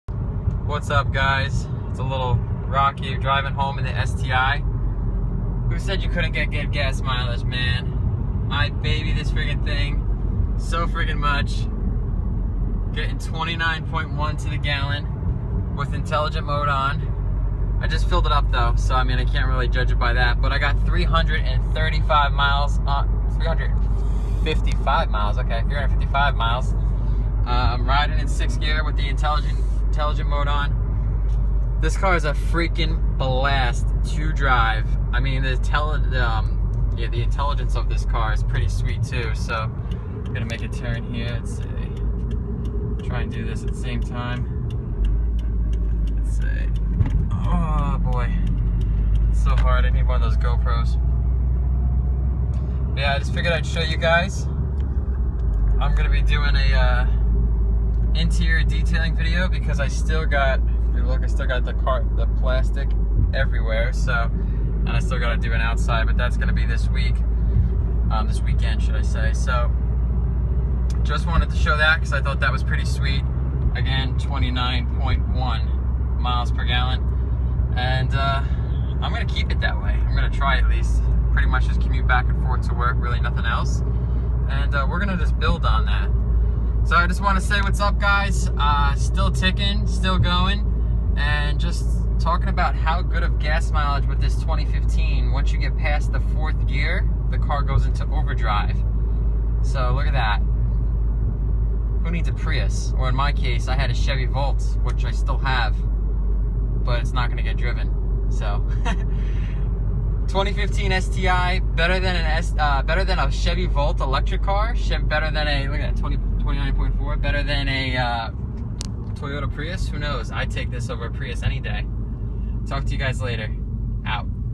2015 Subaru WRX STI Gas Mileage... Bad???
When some people think AWD they think bad gas mileage. When some people think Turbo, they thing bad gas mileage. Well this new 2015 STI now having over drive gets better gas mileage than ever!!! I still own my 2014 Chevy Volt but that will more than likely not get used at all because of this beast of a car!